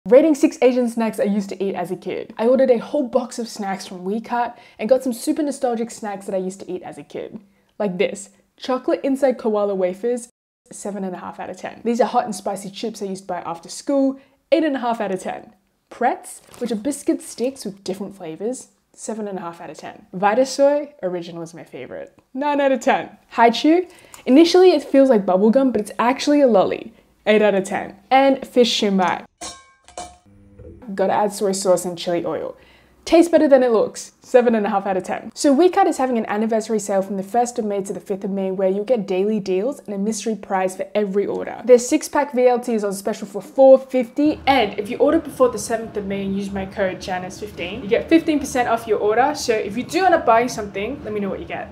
Rating 6 Asian Snacks I Used to Eat as a Kid!
Eating snacks from my childhood bring back all the MEMZ and @wecartaus is having an anniversary sale where you can browse a pretty extensive variety of snacks and relive your childhood 😌 [AD]

Some of my faves are Vitasoy, Yeet Long (Hot & Spicy Chips), Hi-Chew (even though they take forever to chew), Pocky, and those quick and easy steamed dumplings 🥟

They also have some pretty crazy flavour chips, like cucumber, pizza, scallop truffle (this one tastes insaneeeee)

Let me know what you end up getting!